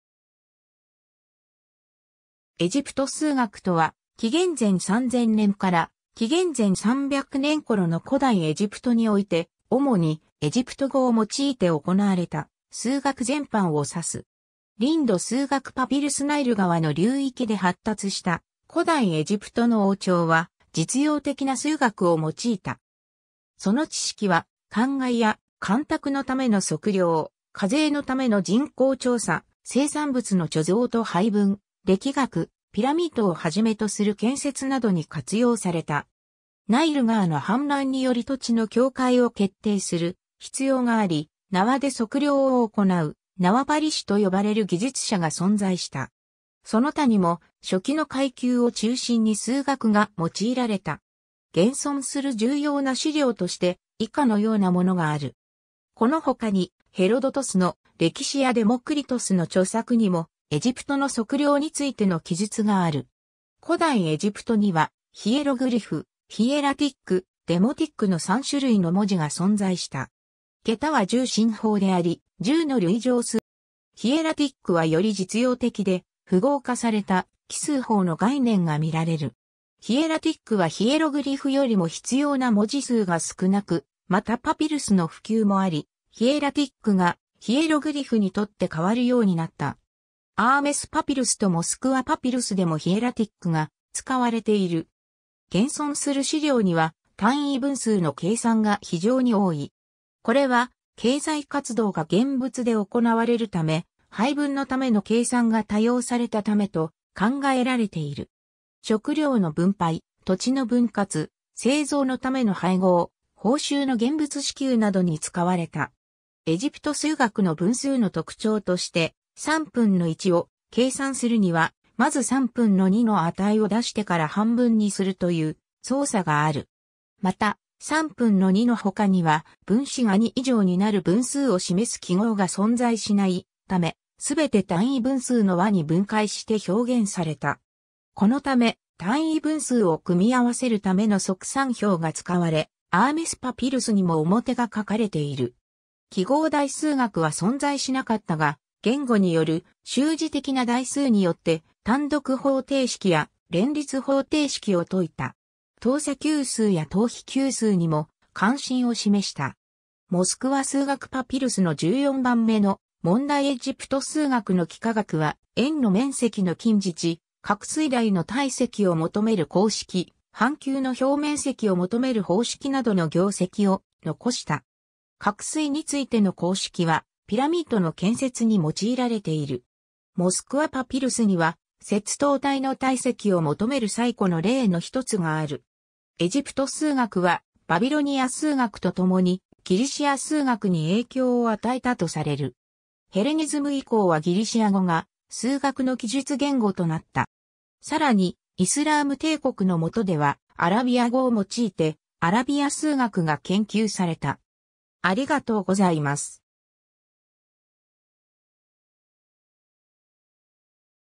エジプト数学
エジプト数学（エジプトすうがく、Egyptian mathematics）とは、紀元前3000年から紀元前300年頃の古代エジプトにおいて、主にエジプト語を用いて行われた数学全般を指す。
 リンド数学パピルス ナイル河の流域で発達した古代エジプトの王朝は、実用的な数学を用いた。
その知識は、灌漑や干拓のための測量、課税のための人口調査、生産物の貯蔵と配分、暦学、ピラミッドをはじめとする建設などに活用された。
ナイル河の氾濫により土地の境界を決定する必要があり、縄で測量を行う「縄張り師」と呼ばれる技術者が存在した。
その他にも書記の階級を中心に数学が用いられた。
 現存する重要な資料として、以下のようなものがある。
 この他に、ヘロドトスの『歴史』やデモクリトスの著作にも、エジプトの測量についての記述がある。
 古代エジプトには、ヒエログリフ（神聖文字）、ヒエラティック（神官文字）、デモティック（民衆文字）の三種類の文字が存在した。
桁は十進法であり、十の累乗数について7乗（一千万）までの絵文字がある。
ヒエラティックはより実用的で、符号化された記数法の概念がみられる。
ヒエラティックはヒエログリフよりも必要な文字数が少なく、またパピルスの普及もあり、ヒエラティックがヒエログリフに取って代わるようになった。
アーメス・パピルスとモスクワ・パピルスでもヒエラティックが使われている。
 現存する資料には、単位分数の計算が非常に多い。
これは、経済活動が現物で行われるため、配分のための計算が多用されたためと考えられている。
食糧の分配、土地の分割、製造のための配合、報酬の現物支給などに使われた。
エジプト数学の分数の特徴として、3分の1を計算するには、まず3分の2の値を出してから半分にするという操作がある。
また、3分の2の他には分子が2以上になる分数を示す記号が存在しないため、すべて単位分数の和に分解して表現された。
このため、単位分数を組み合わせるための速算表が使われ、アーメス・パピルスにも表が書かれている。
 記号代数学は存在しなかったが、言語による修辞的な代数によって単独方程式や連立方程式を解いた。
等差級数や等比級数にも関心を示した。
 モスクワ数学パピルスの14番目の問題 エジプト数学の幾何学は、円の面積の近似値、角錐台の体積を求める公式、半球の表面積を求める方式などの業績を残した。
角錐についての公式は、ピラミッドの建設に用いられている。
モスクワ・パピルスには、切頭体の体積を求める最古の例の1つがある。
 エジプト数学は、バビロニア数学とともにギリシア数学に影響を与えたとされる。
ヘレニズム以降はギリシア語が数学の記述言語となった。
さらにイスラーム帝国のもとではアラビア語を用いてアラビア数学が研究された。